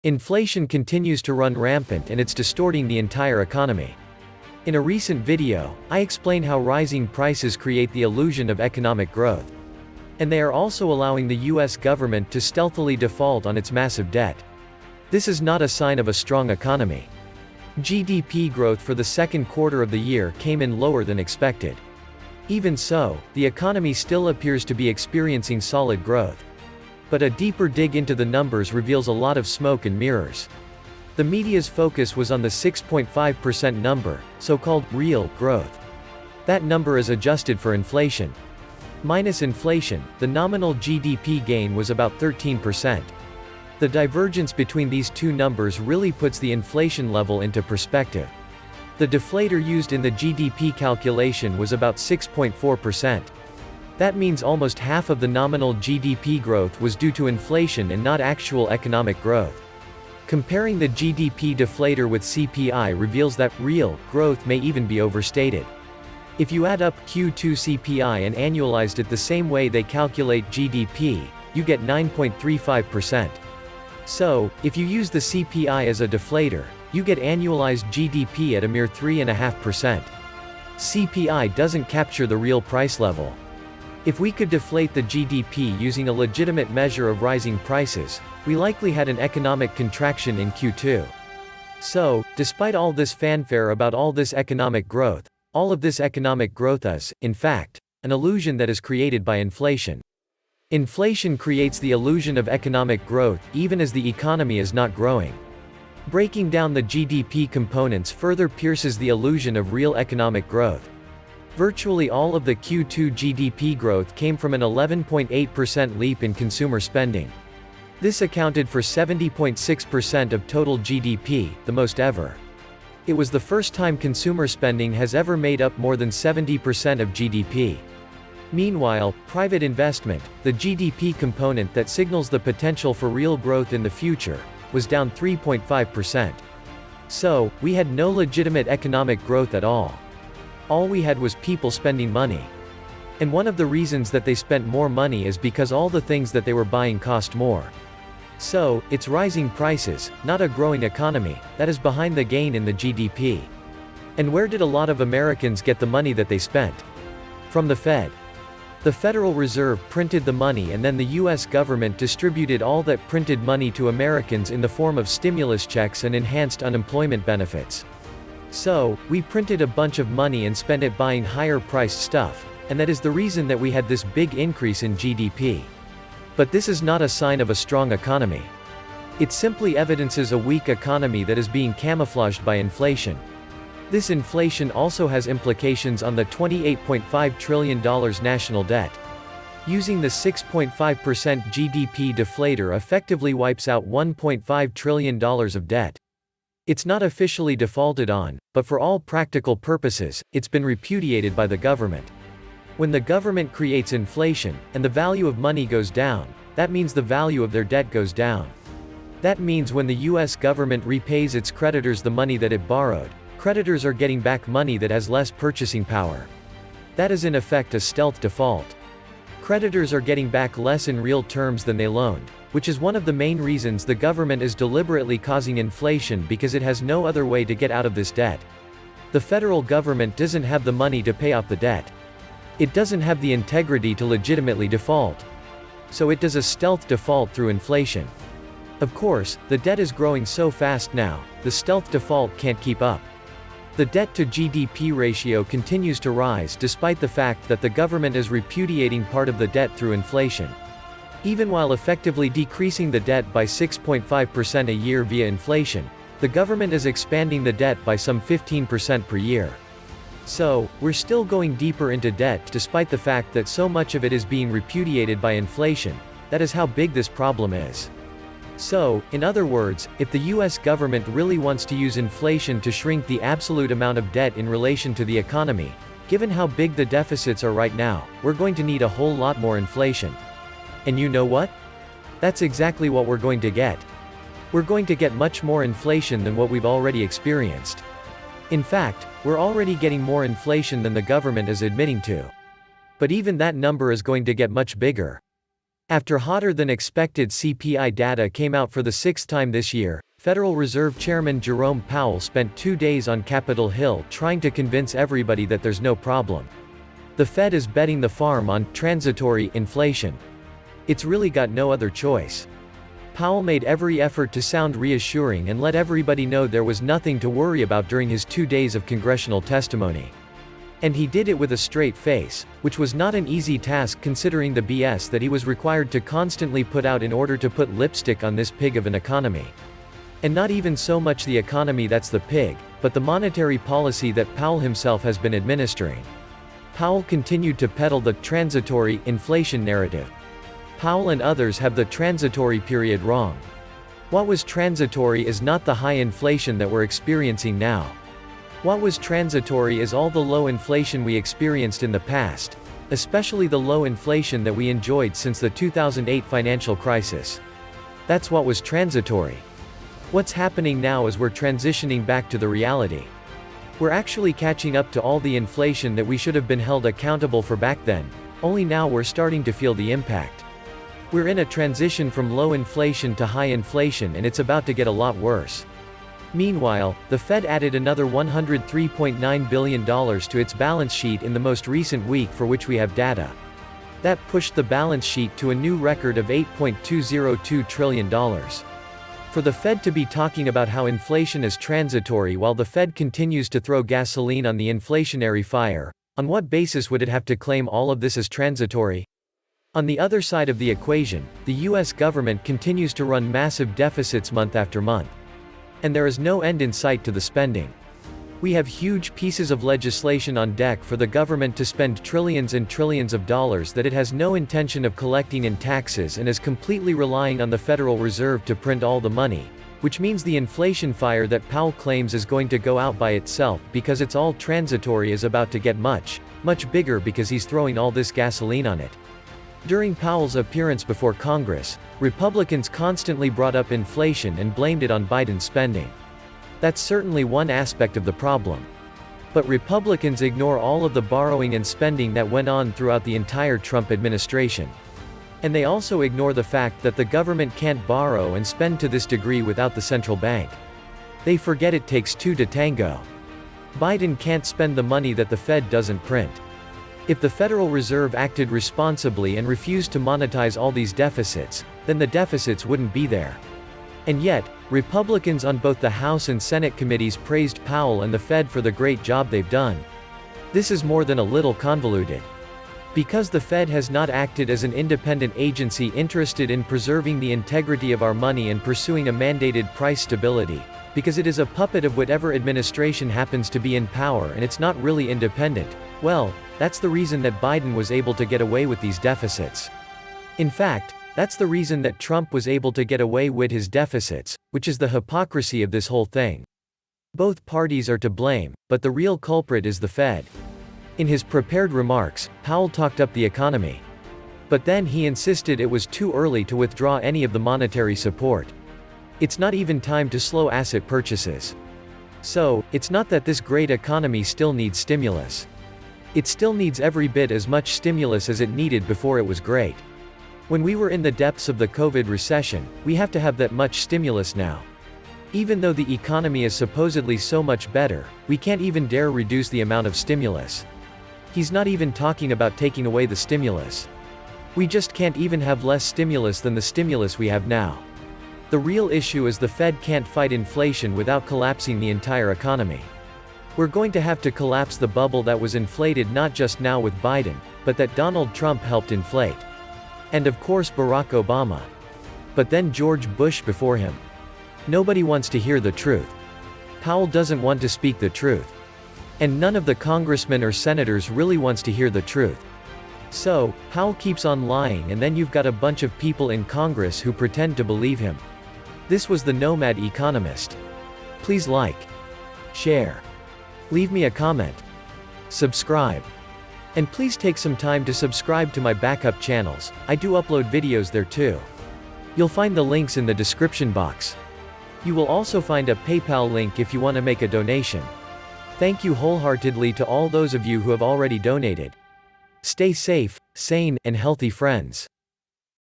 Inflation Soars Distorting The Entire Economy
Inflation continues to run rampant and it’s distorting the entire economy.

In a recent podcast, Peter Schiff explains how rising prices create the illusion of economic growth. And they are also allowing the US government to stealthily default on its massive debt. This is not a sign of a strong economy.